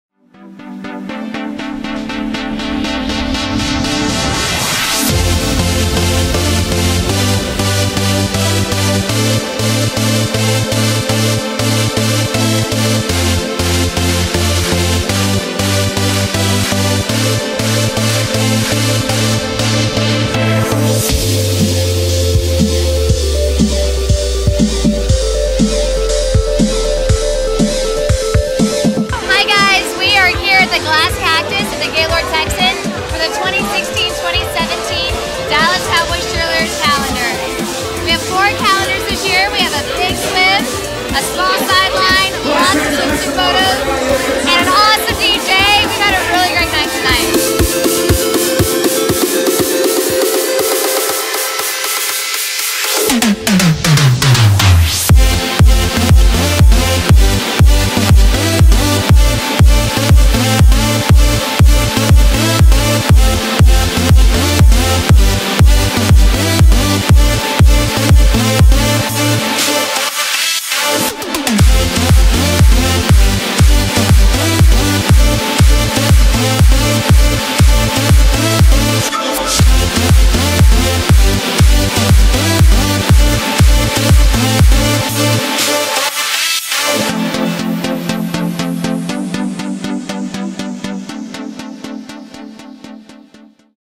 Dallas Cowboy Cheerleaders 2017 Calendar Release Party
The Dallas Cowboy Cheerleaders celebrated the release of their 2017 calendars at the Glass Cactus at the Gaylord Hotel in Grapeviene Texas.